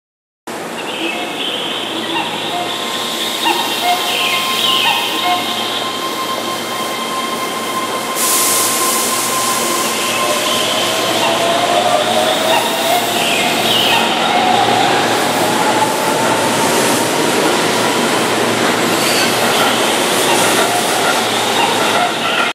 ８/１７ ２回もカモフラージュ入場をして今も車検切れのままの１３０２１編成君は
竣工試運転が２０１４年４月２３日で今年の５月と６且に２回もカモフラージュ入場をして今も車検切れのままの１３０２１編成君は・・ことらはＫ０７０６Ｚの便になりまして
今日は最終がＧ１８０４Ｙのスジの仕事を貰ってます（汗）